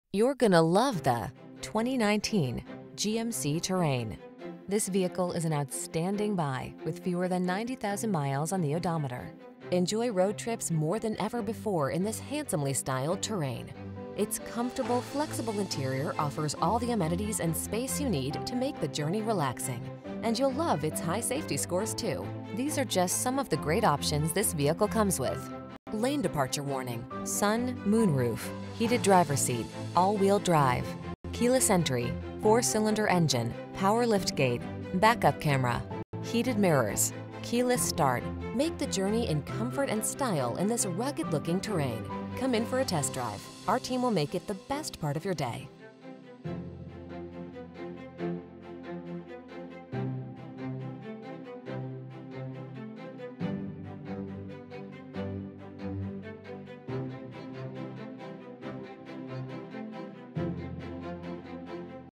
2019 GMC Terrain SLE DE Wilmington, New castle, Newark, Hockessin, Claymont
Pre-Owned 2019 GMC Terrain SLE available at Diver Chevrolet located in 2101 Pennsylvania Ave, Wilmington, DE, 19806. Servicing the Wilmington, New castle, Newark, Hockessin, Claymont area.

You're gonna love the  2019  GMC Terrain.  Enjoy road trips more than ever before in this handsomely styled Terrain. Its comfortable, flexible interior offers all the amenities and space you need to make the journey relaxing. And you'll love its high safety scores, too! These are just some of the great options this vehicle comes with: Lane Departure Warning, Sun/Moonroof, Heated Driver Seat, All Wheel Drive, Keyless Entry, 4 Cylinder Engine, Power Liftgate, Back-Up Camera, Heated Mirrors, Keyless Start. Make the journey in comfort and style in this rugged-looking Terrain. Come in for a test drive. Our team will make it the best part of your day.